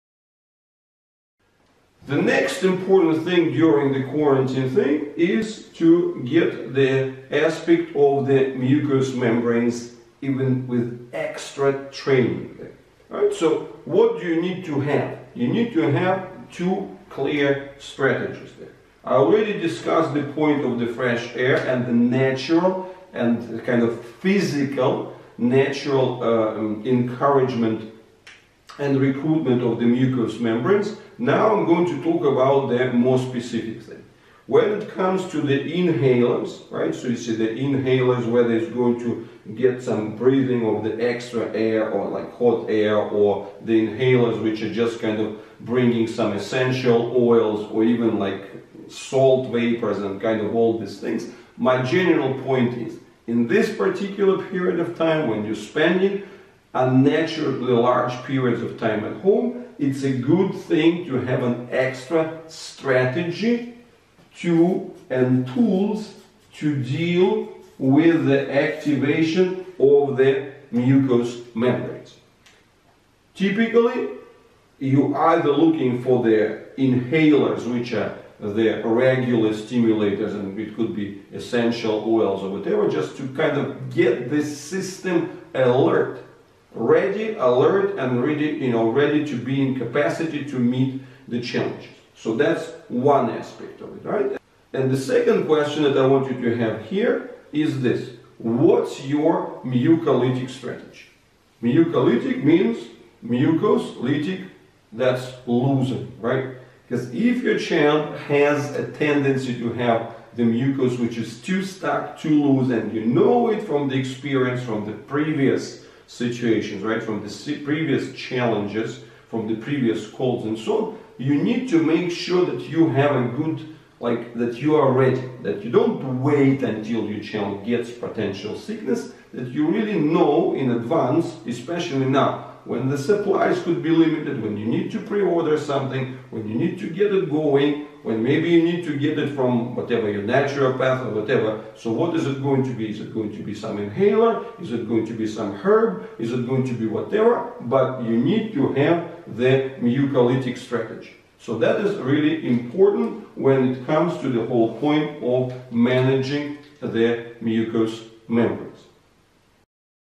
Cerebral palsy, Respiration and Coronavirus COVID19 What is your mucous membranes strategy?
Next step in Respiratory alertness for cerebral palsy! In times of uncertainty and an elevated risk for fragile children with cerebral palsy, it is really helpful to think ahead and to have pre-installed strategies in place. Our mucous membranes within the airways and respiratory system are our first line of defense against all pathogens, viruses included. If these membranes are moist, active and timely cleared -- then the risks of illness reduces significantly. This short video is just a reminder that you need to speak to your doctor or a naturopath and find the right tools that fit your child. It is better to do this ahead as a prevention, rather than an afterthought as a treatment.